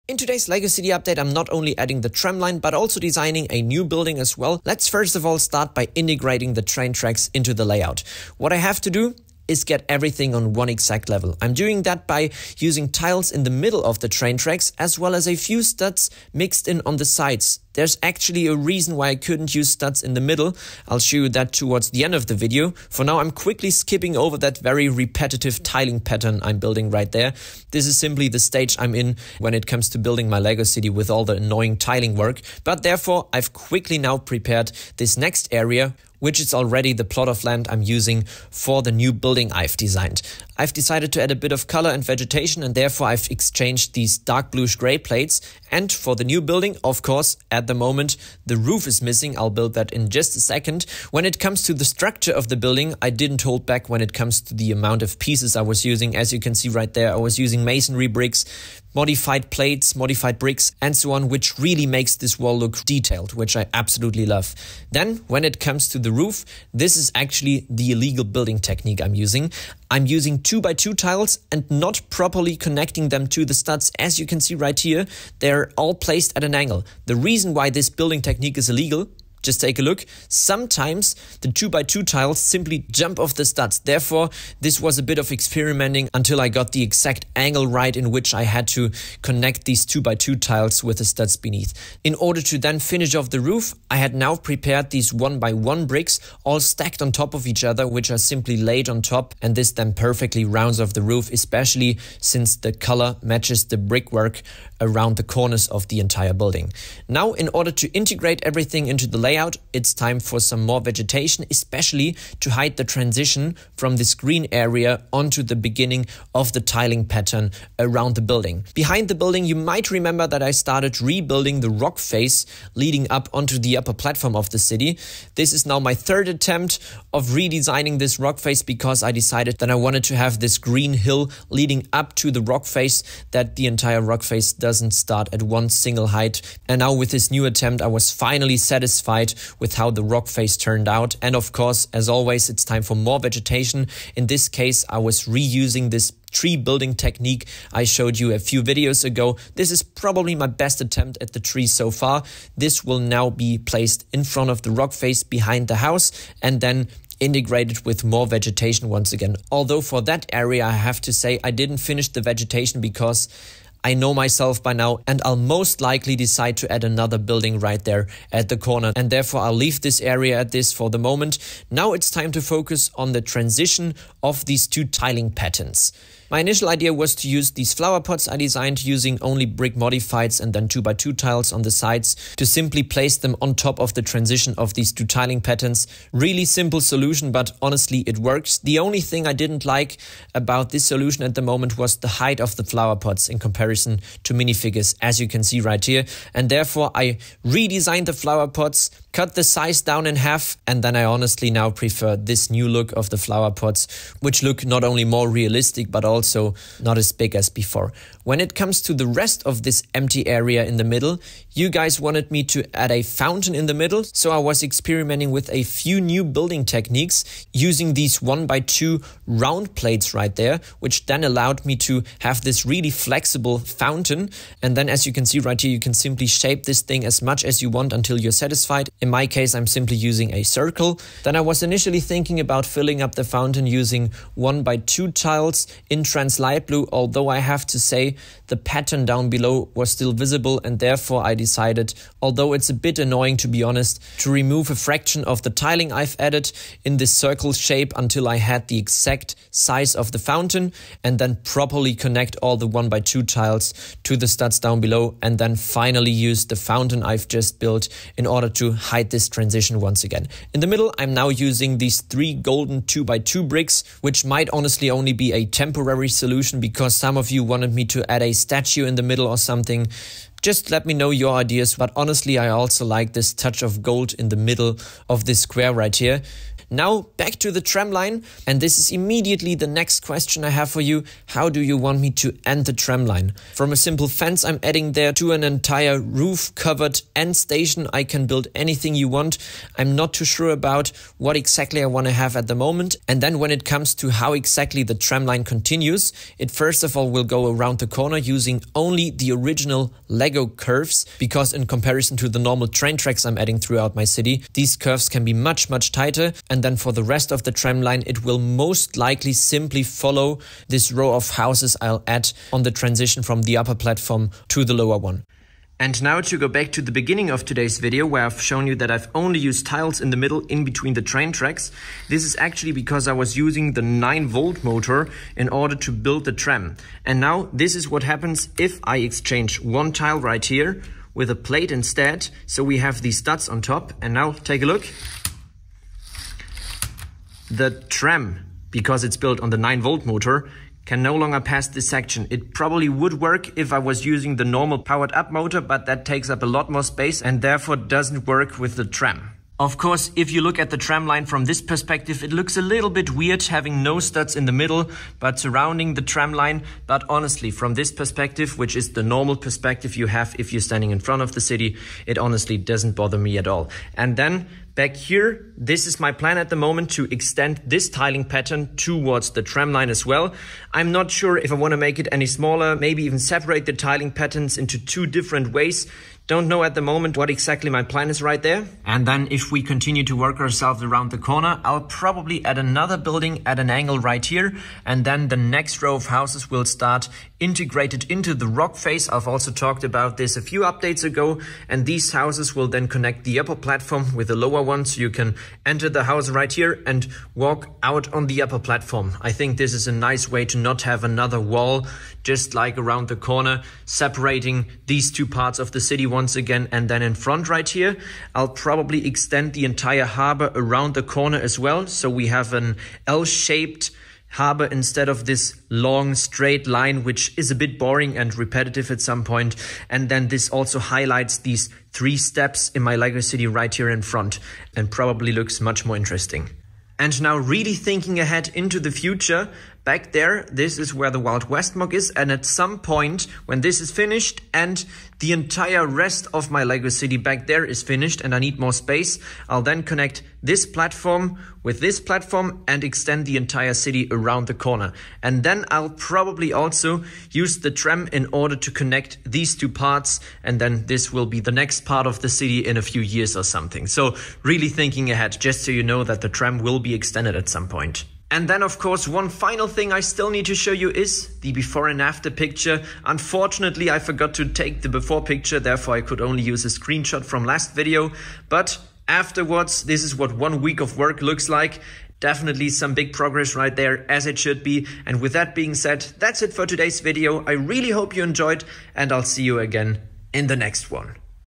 Adding the Tram Line... - LEGO City Update
In today's LEGO City Update it's time for the tram to be installed!